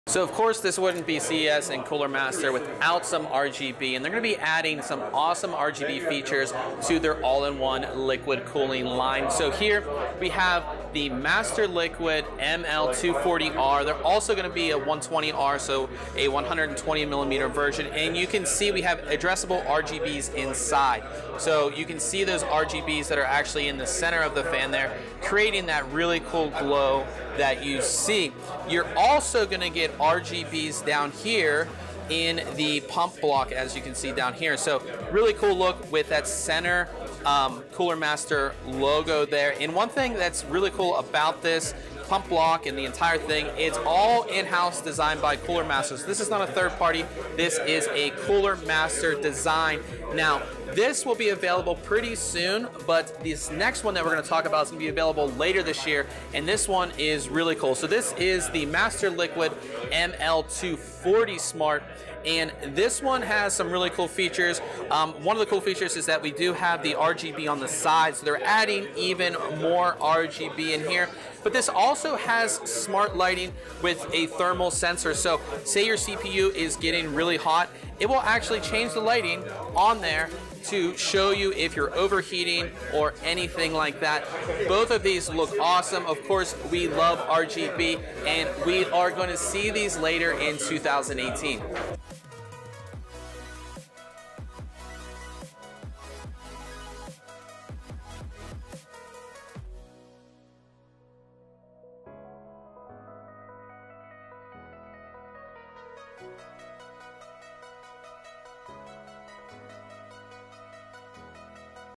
Cooler Master's RGB Liquid CPU Coolers For 2018
Cooler Master's RGB Liquid CPU Coolers For 2018, the MasterLiquid ML120R, ML240R, and ML240 Smart AIO Liquid Coolers.